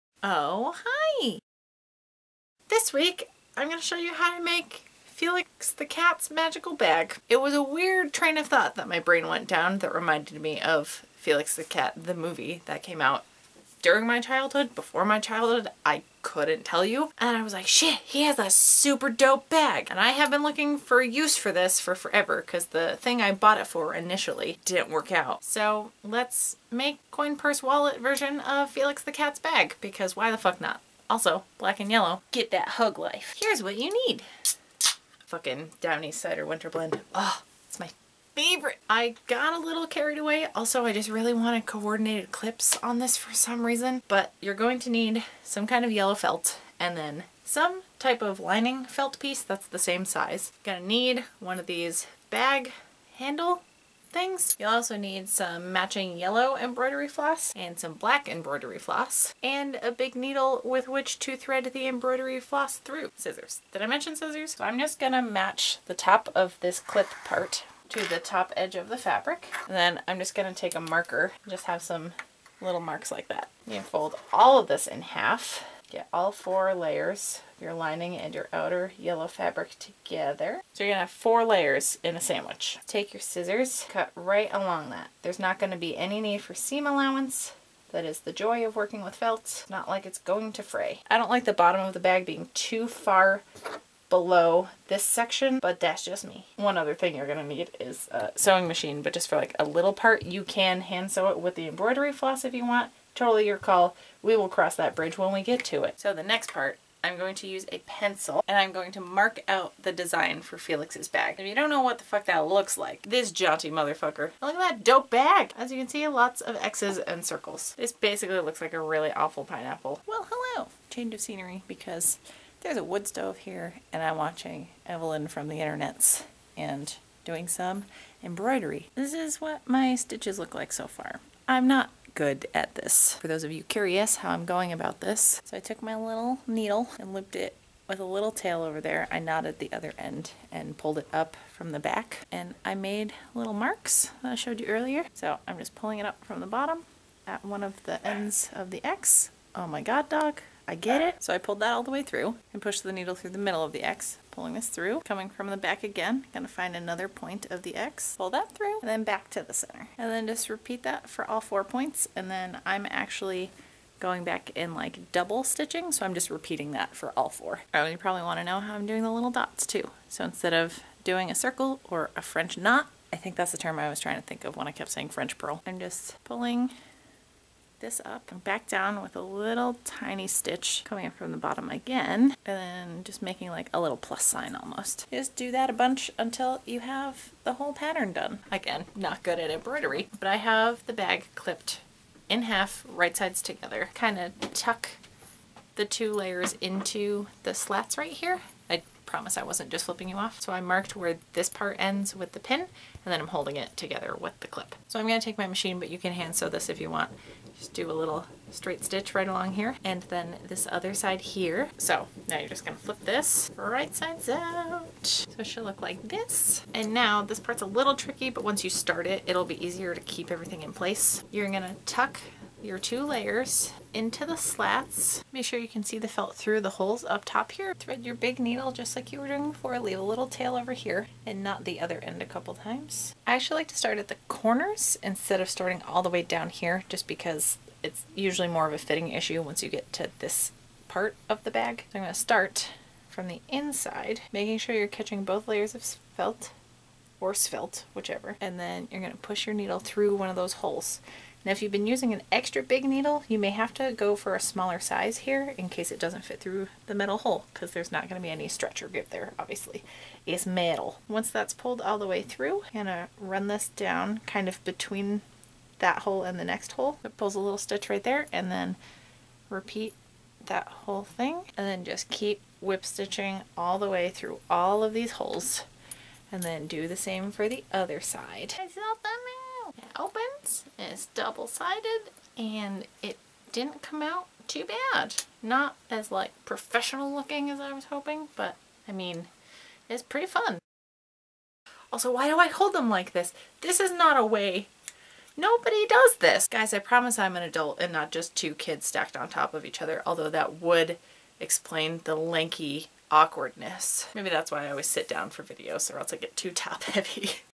TUTORIAL! | Felix the Cat's Bag! | Sewing Nerd!
Hi guys!
This week I show you how I made the magic bag from Felix the Cat!
I don't know if anybody else saw the movie growing up, or the old cartoon show.
Anyways, this was a fun one to throw together!

Cheers!